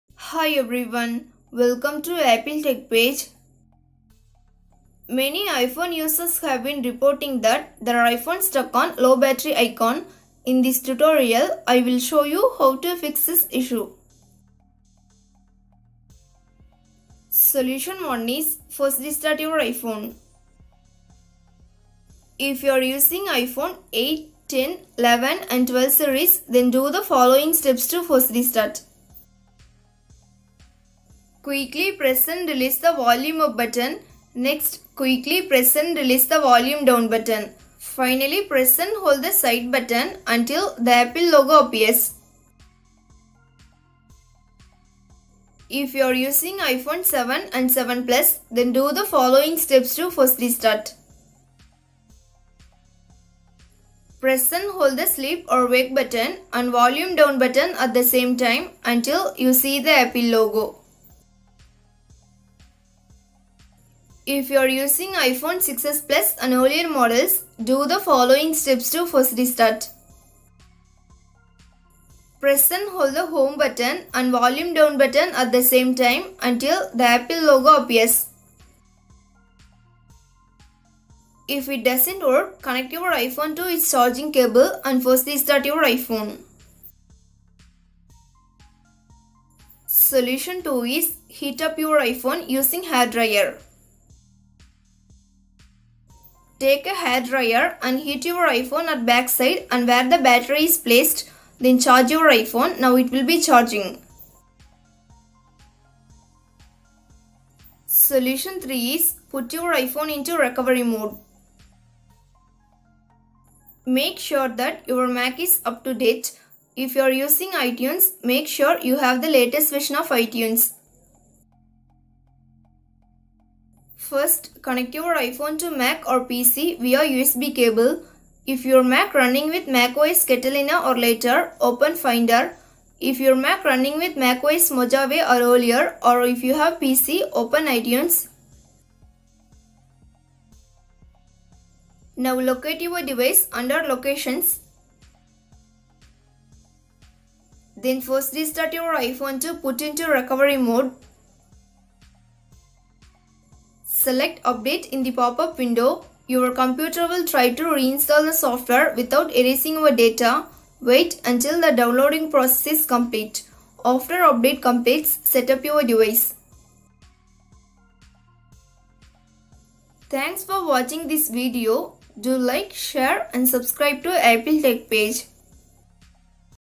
iPhone or iPad Stuck on Red/Low Battery Icon in iOS 14.5 [Fixed]
iPhone stuck on red charging screen in iOS 14.5.1, iPhone stuck on red battery logo screen, iPhone 12 pro max stuck on red battery screen, iPhone 11 stuck on low battery icon.